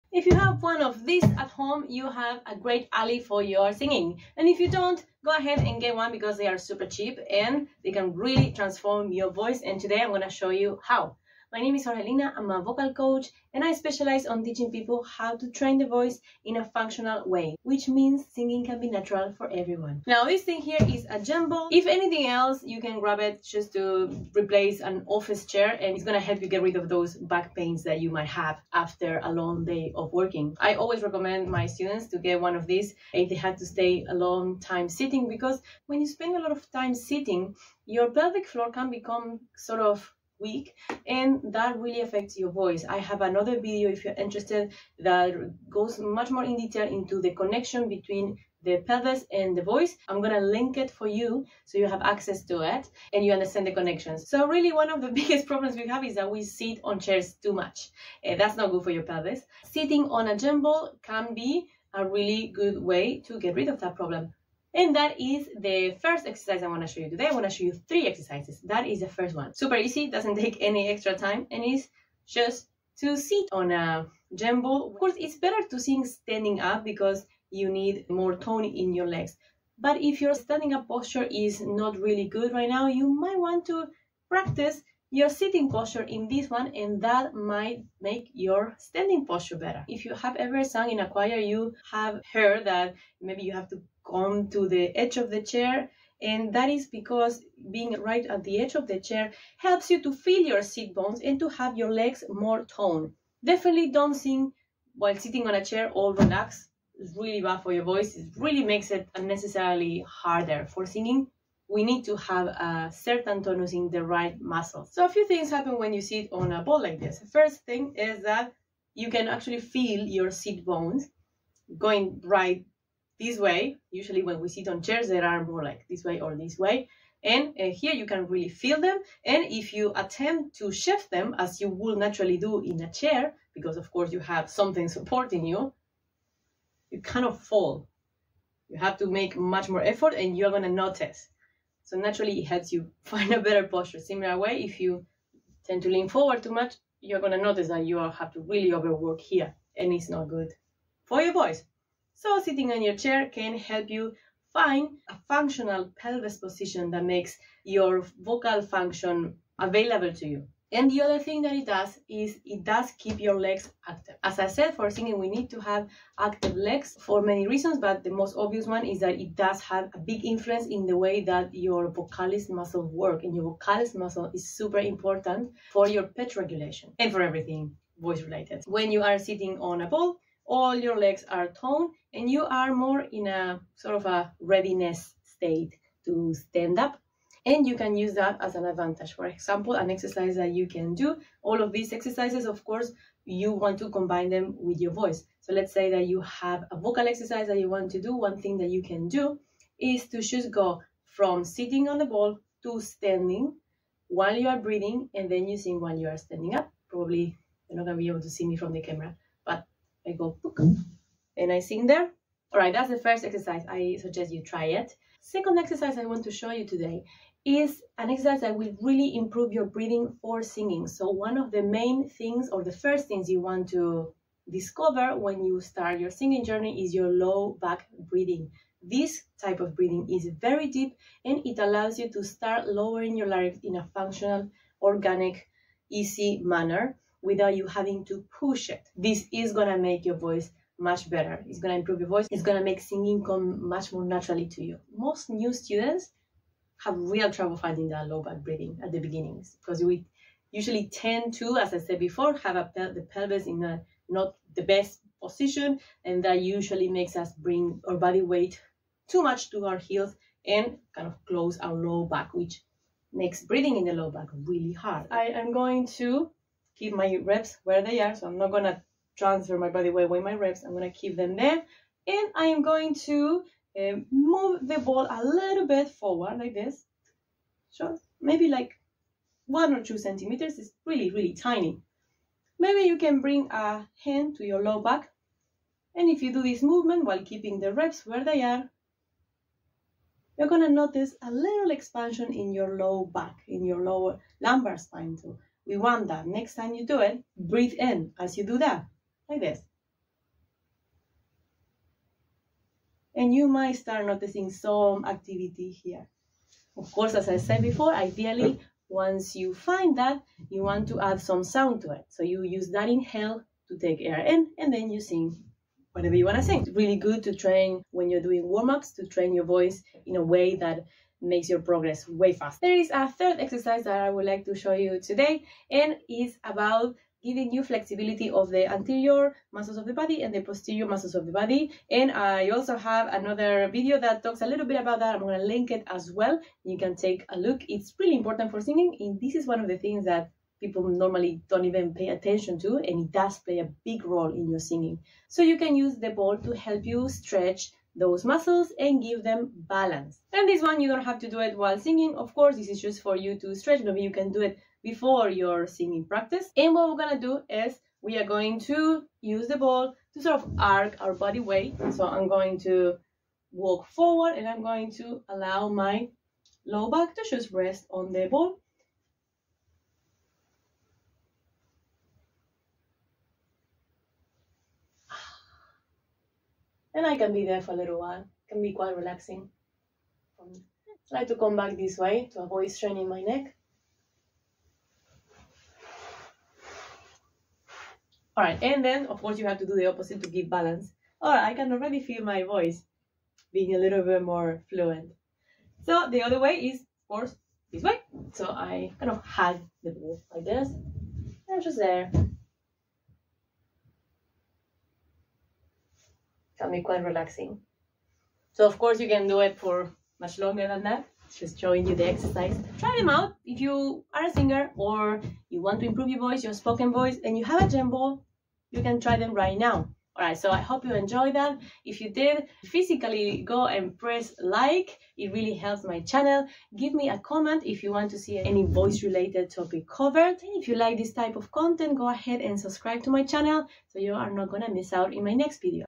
3 Gym Ball Exercises That Will Transform Your Voice Today!
GRAB MY 3 SUPER GENTLE VOCAL WARM UPS FOR TIRED VOICES!

Did you know you can use your gym ball to transform your voice?
Let me tell you, it can be your new vocal secret weapon!
In this video, I show you my favourite 3 gym ball exercises that you can use right now to transform your voice. Enjoy!

00:00 Intro
00:30 Using a gym ball everyday to improve your pelvic floor
01:08 Sitting on a gym ball to improve your voice
03:35 Using a gym ball to expand your low-back breathing
05:23 Increasing flexibility and balance of anterior and posterior postural muscles for optimal voice production

Just click on the LIFETIME or YEARLY option and in the checkout card add code 25OFF

EXPLORE MY PROGRAMS!